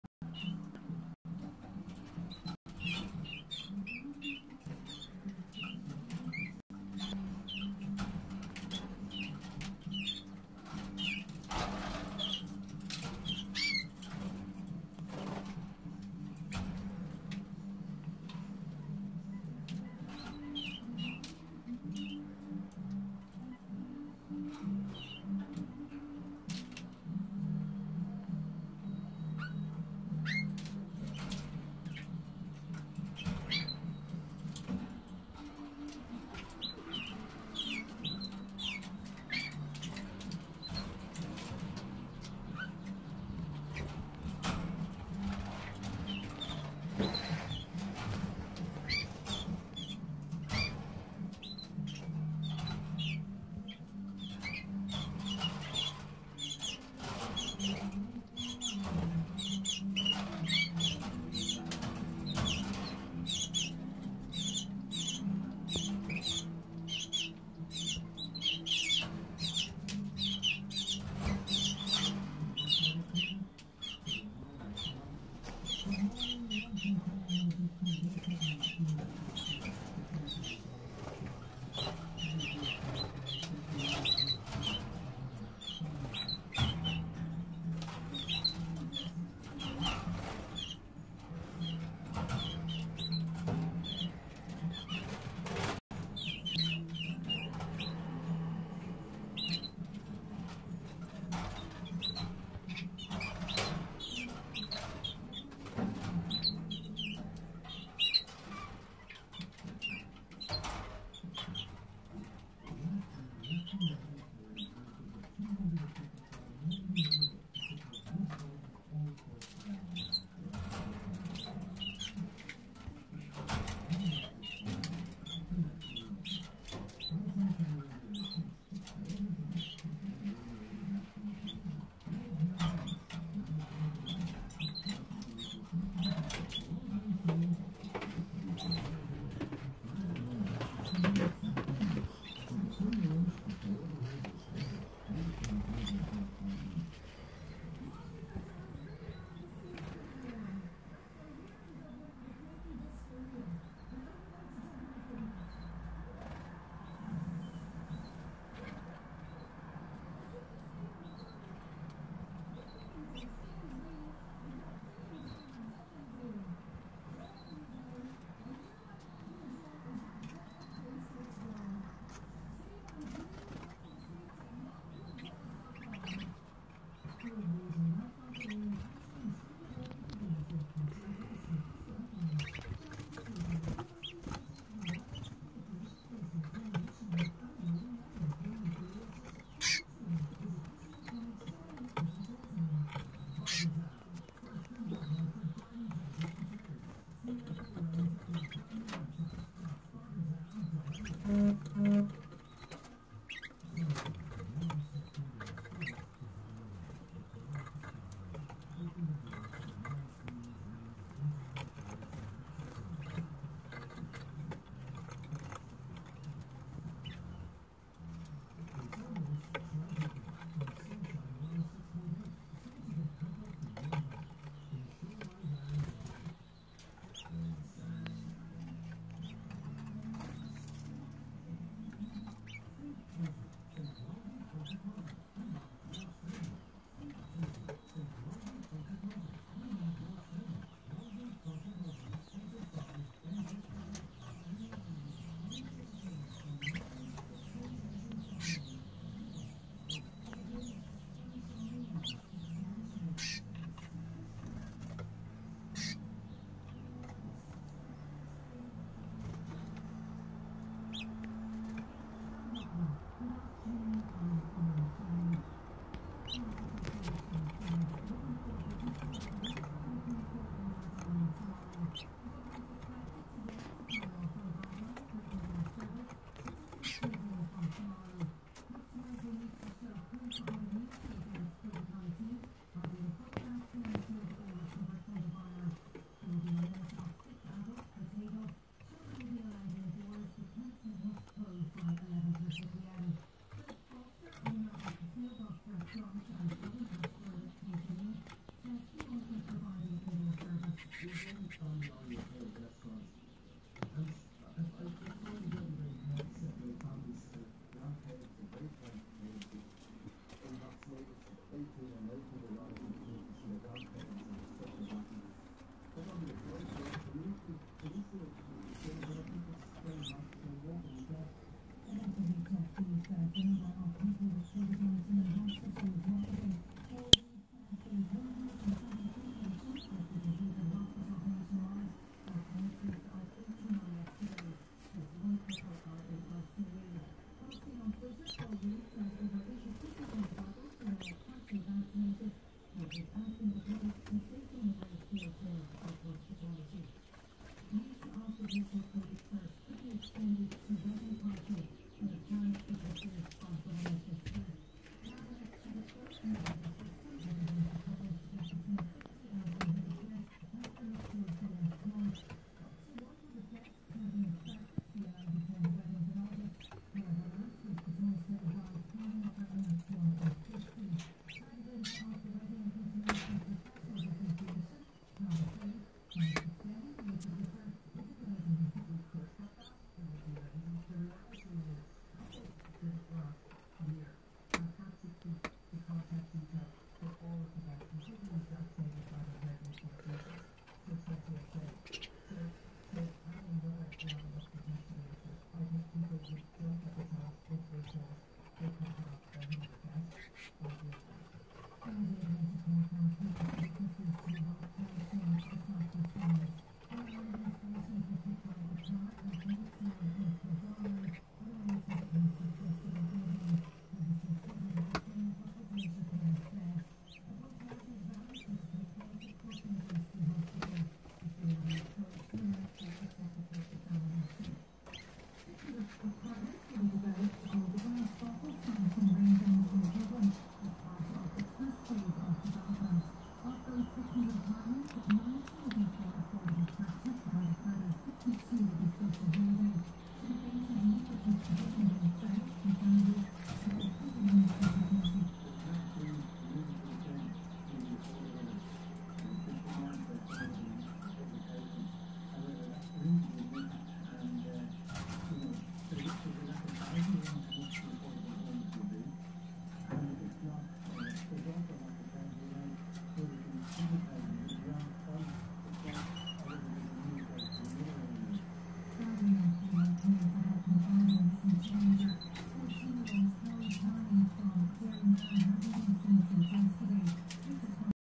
canarys Ireland July 25 2021
breeding season comming to a end in the canary sheds